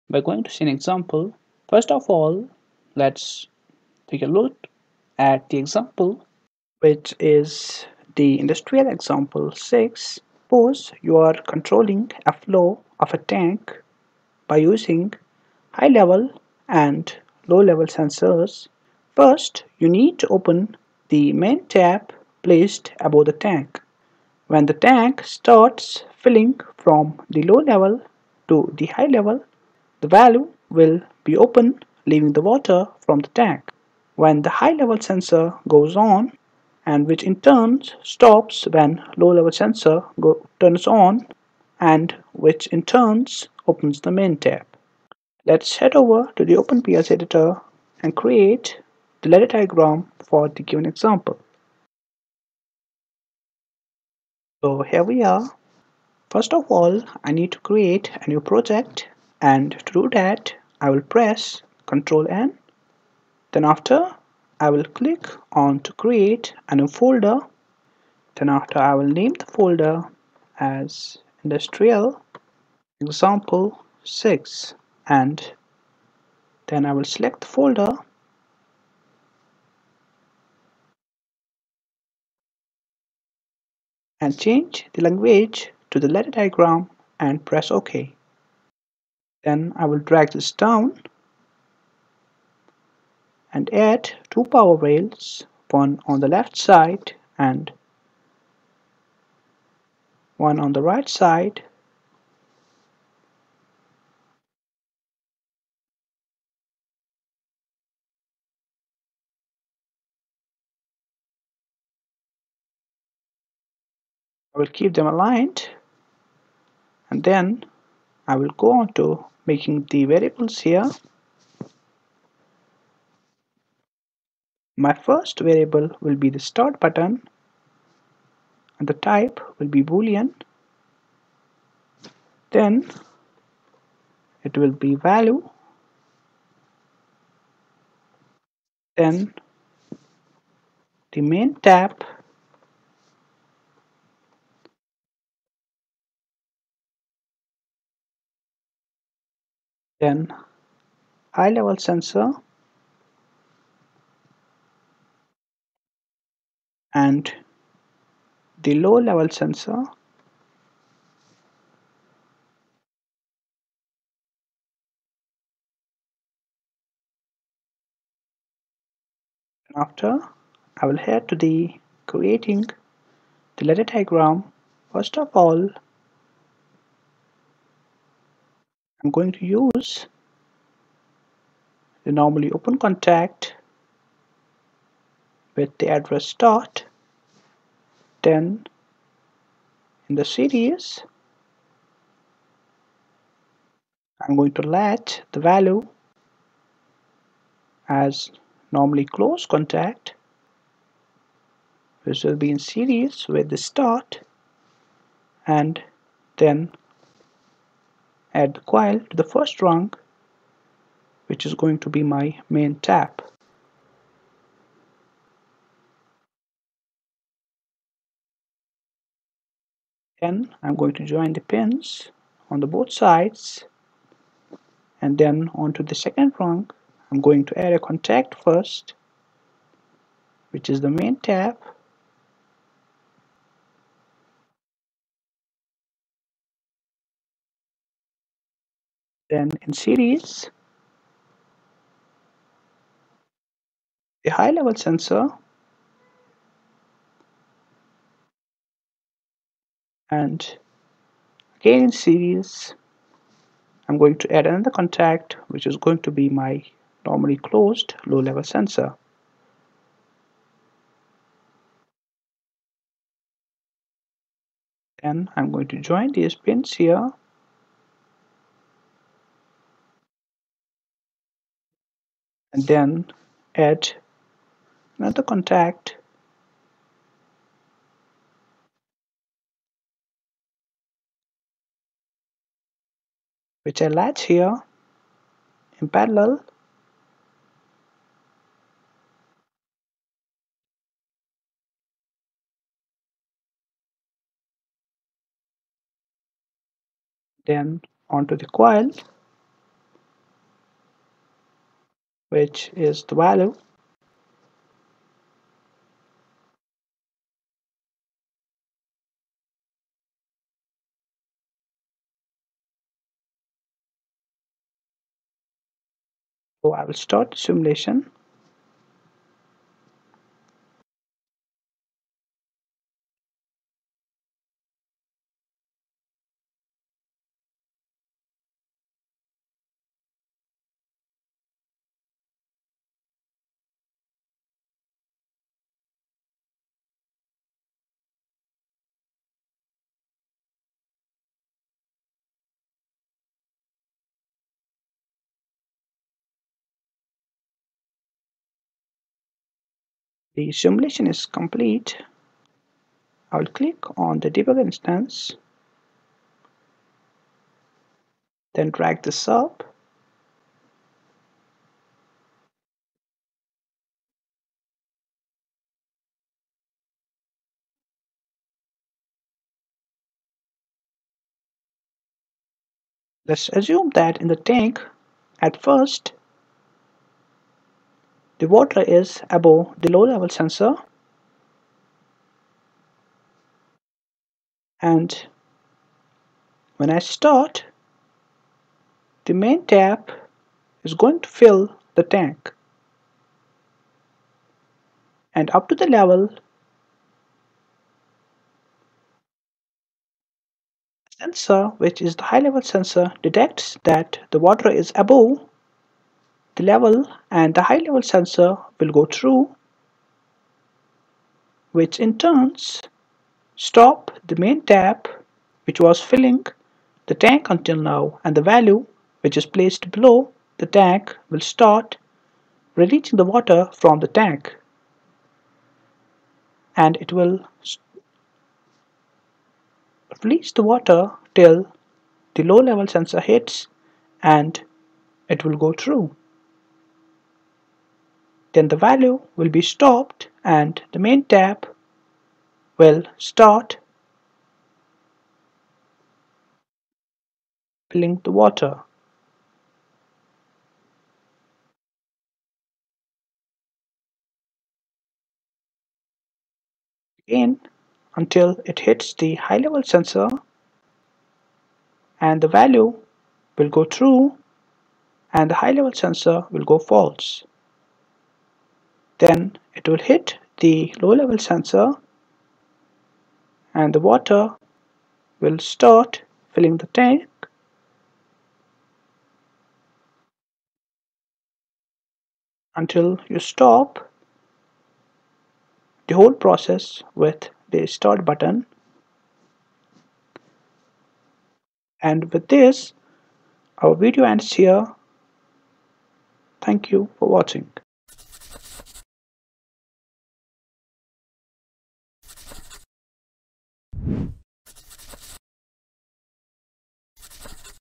Simple PLC Program - Control Flow Through a Tank
In this video, you will learn the simple PLC program which is to control flow through a tank.